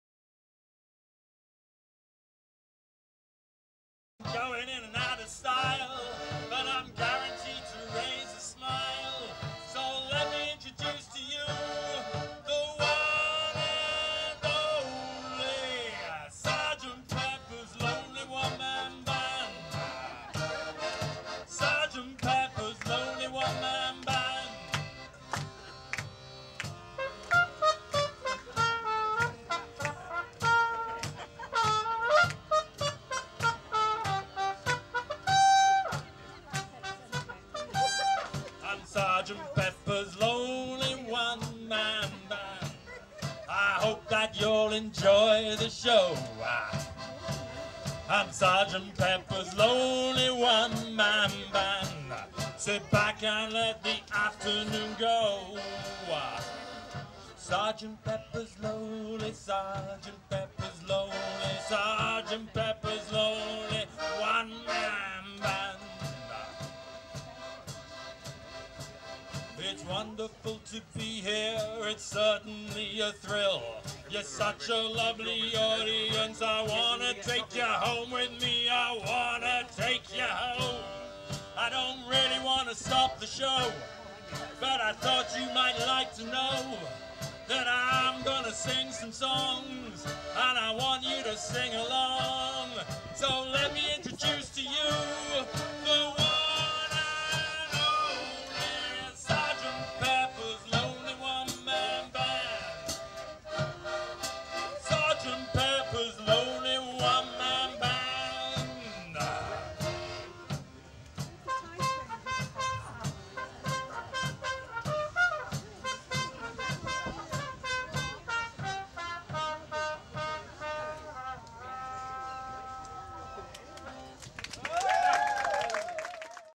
Pete Moser | One Man Band
Pete is a singer and instrumentalist playing accordion, piano, trumpet, percussion, guitars and whistles, with four different one man band personas to choose from that are guaranteed to entertain.